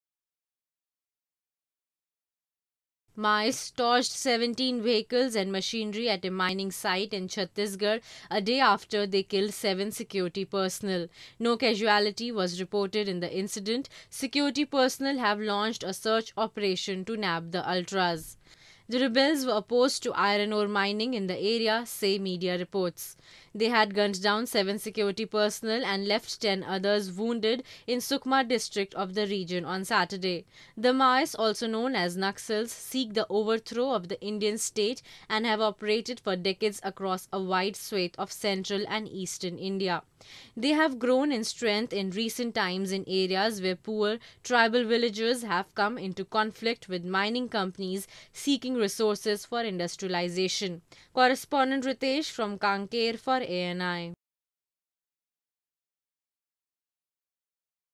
Maoists strike again, torch 17 vehicles carrying iron ore in Chhattisgarh
Kanker, April 13 (ANI): Maoists torched 17 vehicles and machinery at a mining site in Chhattisgarh, a day after they killed seven security personnel. No casualty was reported in the incident. Security personnel have launched a search operation to nab the ultras.The rebels were opposed to iron ore mining in the area, say media reports. They had gunned down seven security personnel and left ten others wounded in Sukma district of the region on Saturday. The Maoists, also known as Naxals, seek the overthrow of the Indian state and have operated for decades across a wide swathe of central and eastern India. They have grown in strength in recent times in areas where poor, tribal villagers have come into conflict with mining companies seeking resources for industrialisation.